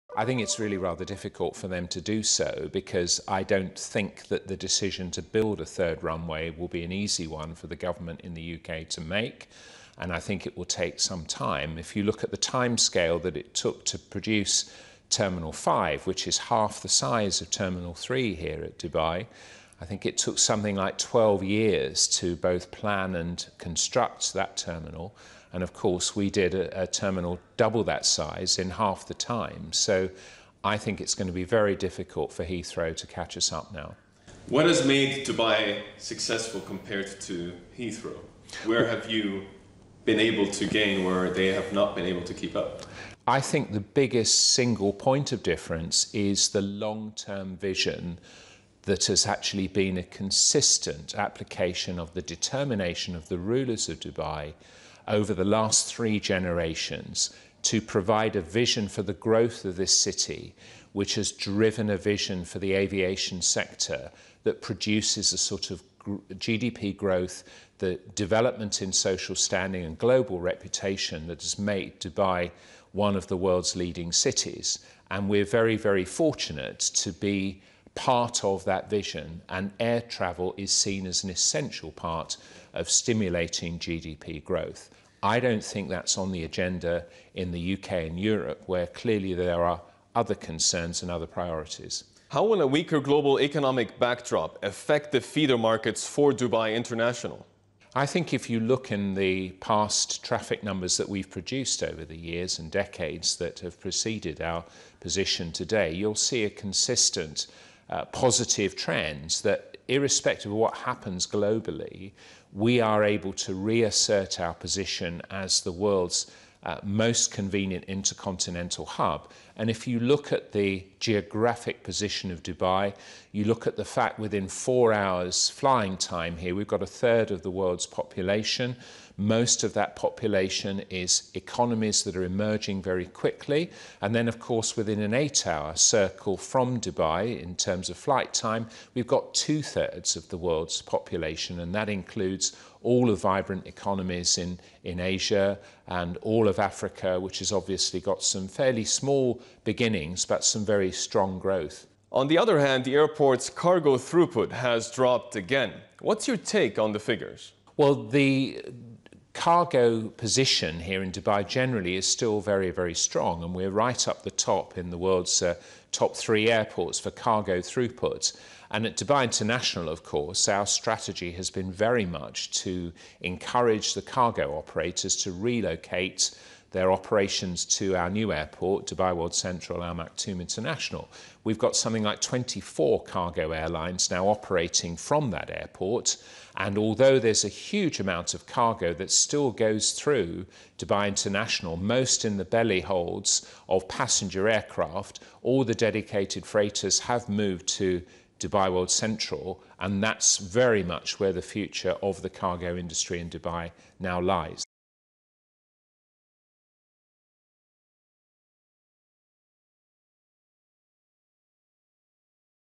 Dubai Airport takes Heathrow's Crown | Dubai Airport CEO Exclusive | CNBC International
As Dubai Airport takes Heathrow's crown as the busiest international hub, CNBC speaks to Dubai Airports CEO.

Heathrow has been dethroned by Dubai International as the busiest international air hub. Paul Griffiths, CEO of Dubai Airports, says that "long term vision" is the key to Dubai's city growth and therefore its air traffic.